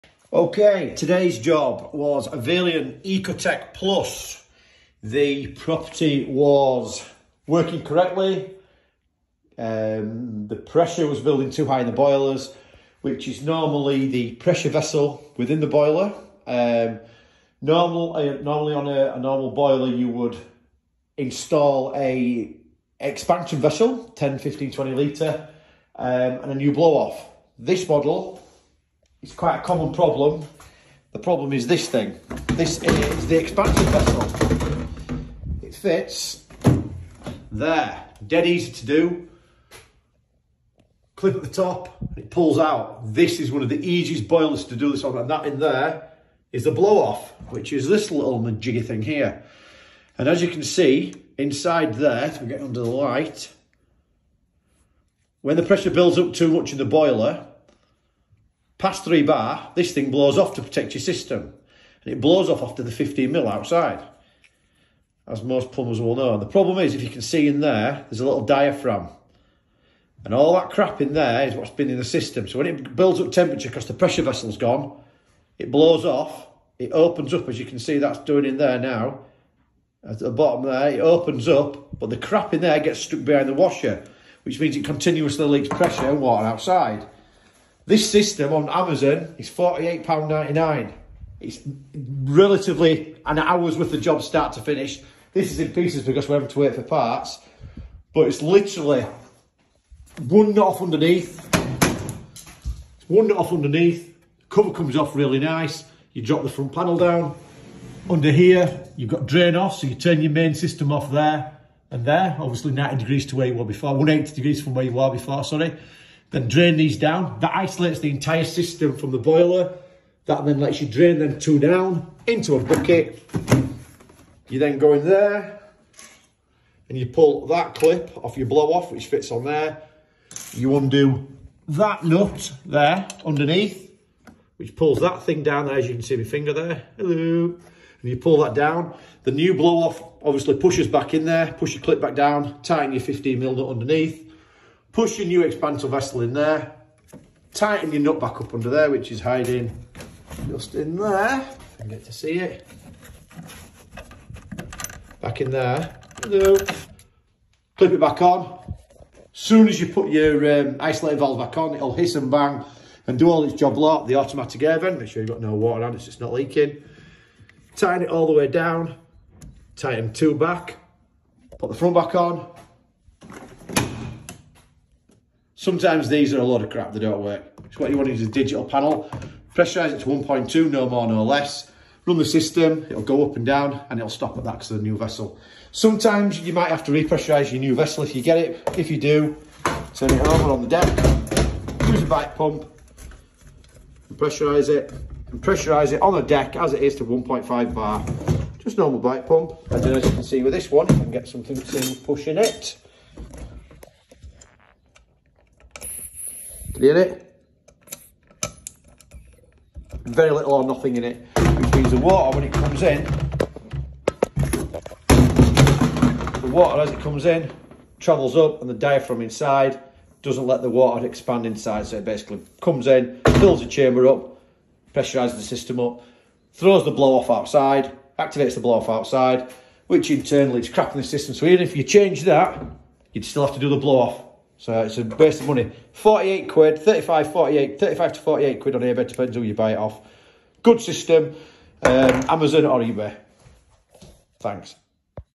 Vailiant Ecotech plus boiler pressure problem
Boiler was having over pressure problems
And blowing off outside. Message me and I can supply a full kit to fix the problem,